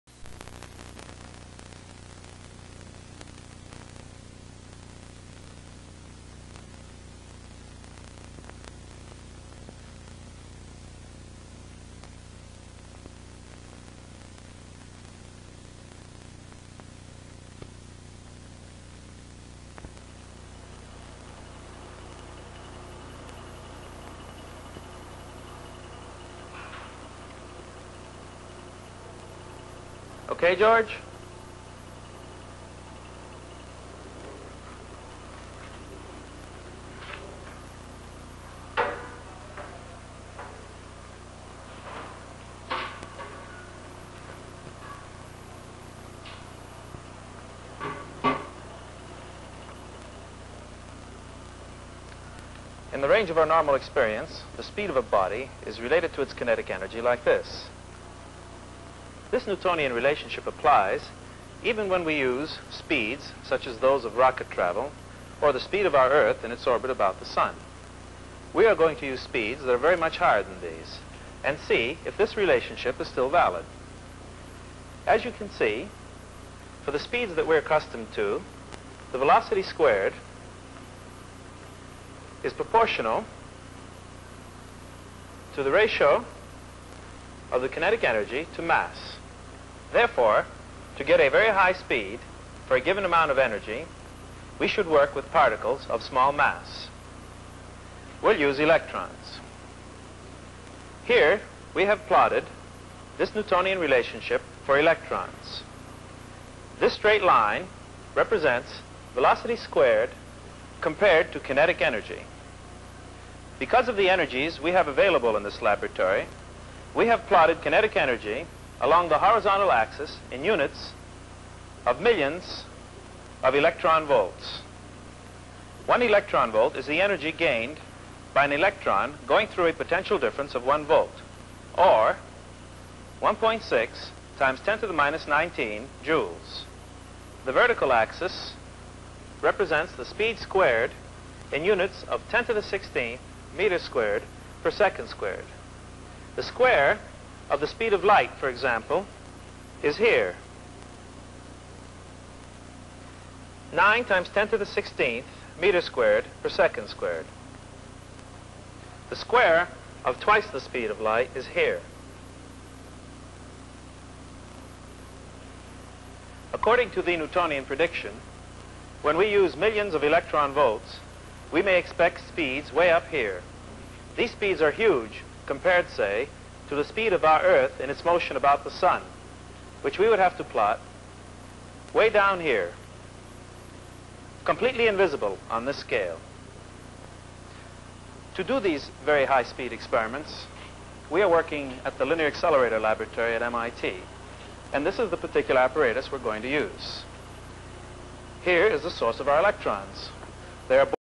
Relativity and Apparent Mass
The first few minutes of an educational video from the 1960s that explains the apparent increase in mass of an electron at relativistic speeds.